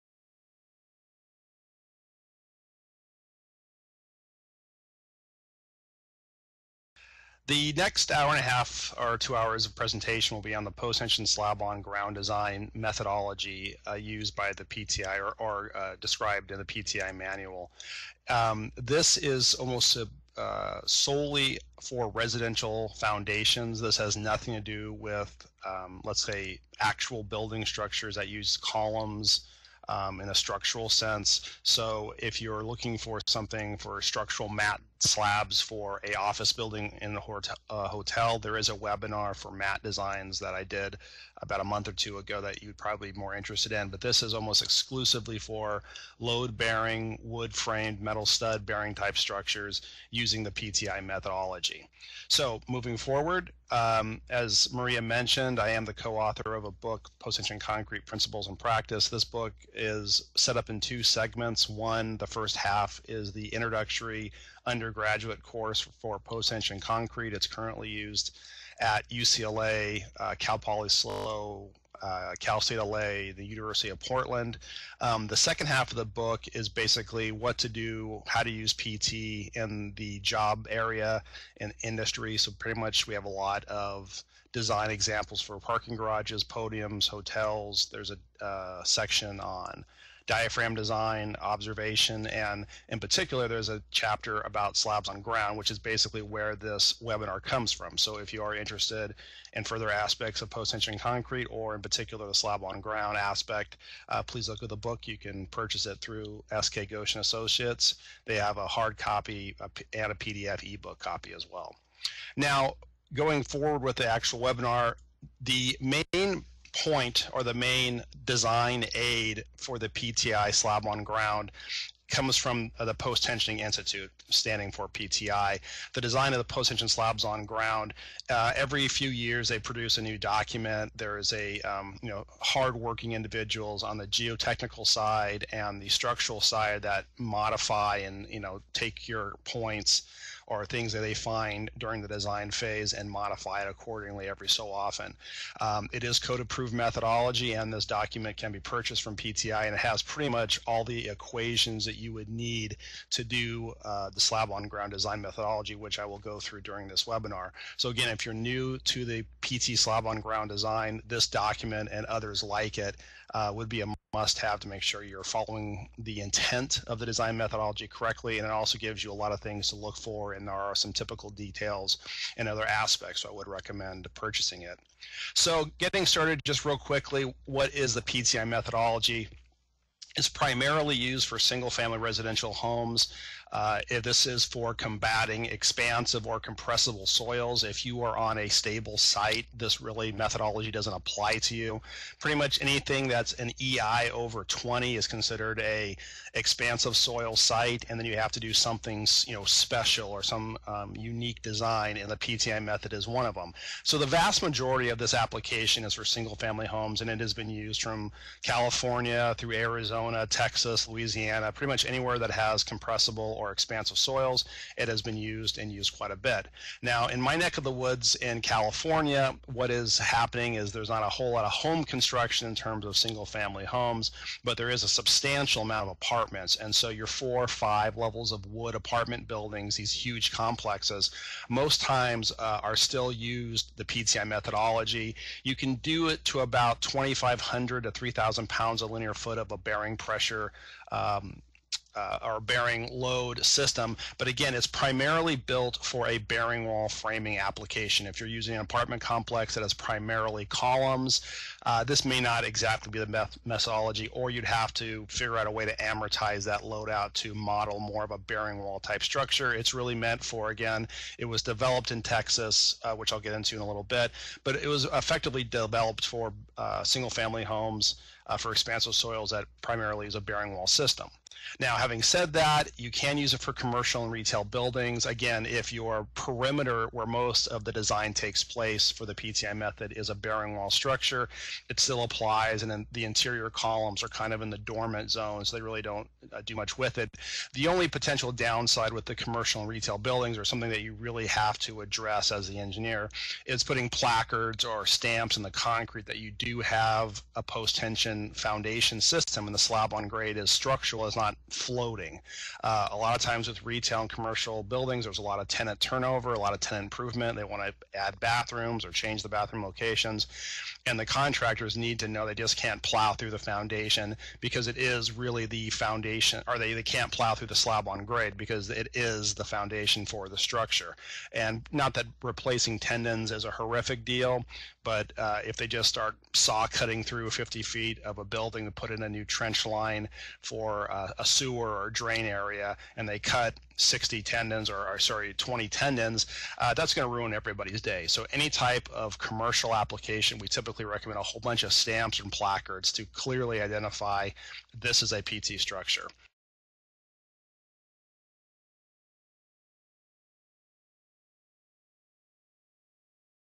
Design and Construction of Post-Tensioned Slabs on Ground Using the PTI Method
The webinar covers the engineering and construction aspects of post-tensioned slab on ground foundations for residential buildings that are used to resist the movement of expansive and compressible soils. Construction photographs in conjunction with numerical examples are used to explain the intended use of the PTI methodology. In addition, photographs highlight several issues to avoid during the design and construction. A working knowledge of the PTI method is not required.